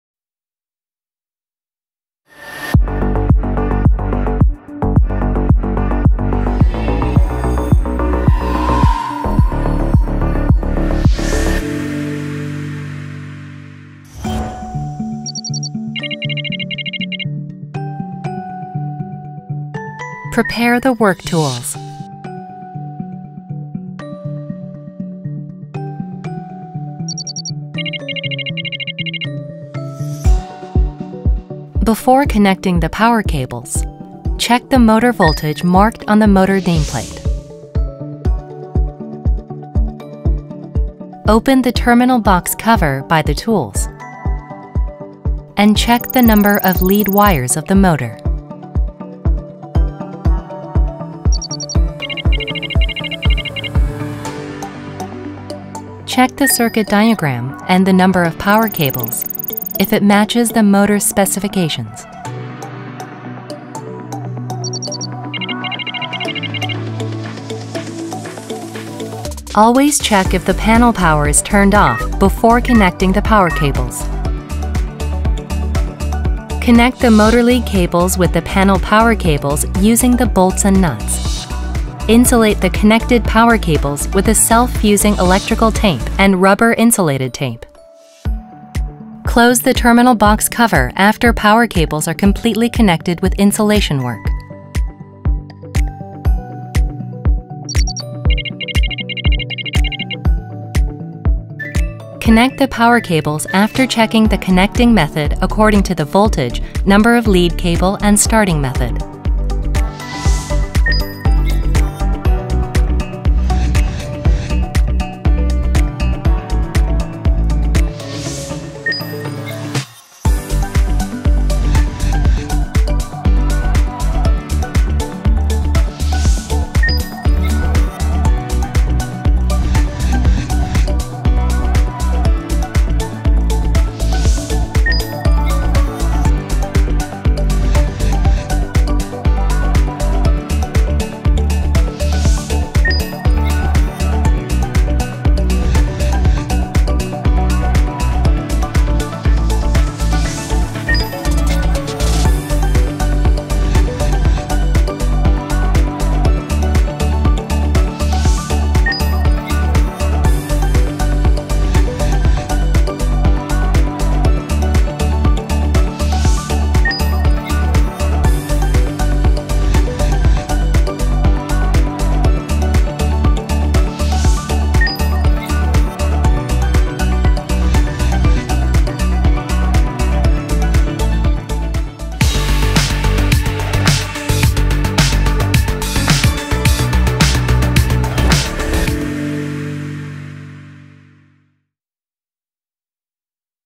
[HOW-TO] Connect the Low Voltage Induction Motor by each specification
This video is to inform how to connect the power cables for 3phase induction motor by each specification.